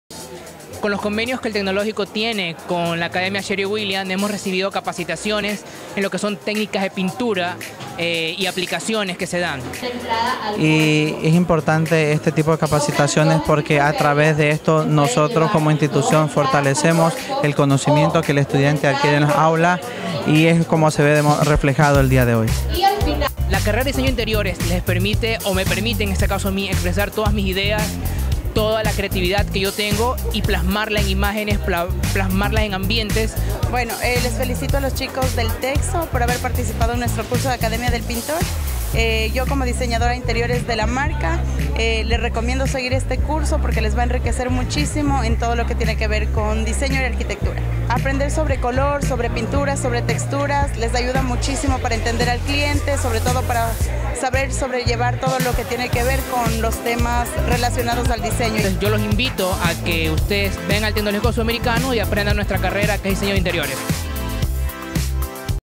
Academia del Pintor Sherwin Williams
Estudiantes y Docentes de la Carrera de Diseño de Interiores, recibieron certificación por parte de la Universidad Internacional del Ecuador en convenio con la empresa Sherwin Williams, luego de haber finalizado con éxito los módulos de capacitación en acabados, texturas, colores y pintura, recibido en el programa la Academia del Pintor.